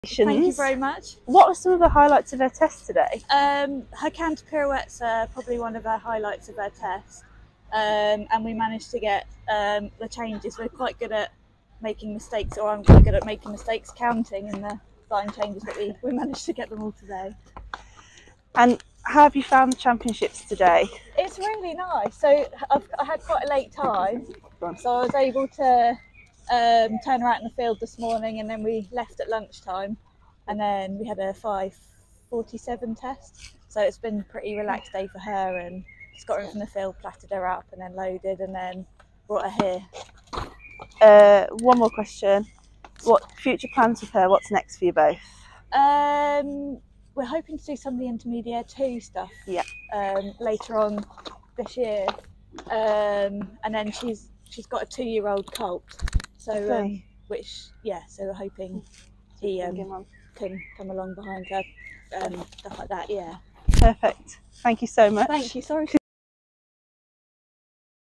Petplan Winter Area Festival Intermediate I Silver Champion 2024: Julia Walker and Rodhea
Julia Walker and her own Rodhea topped the Petplan Intermediate I Silver Winter Area Festival Championship with 66.029% as the second day of the championships came to a close in Petplan Arena One.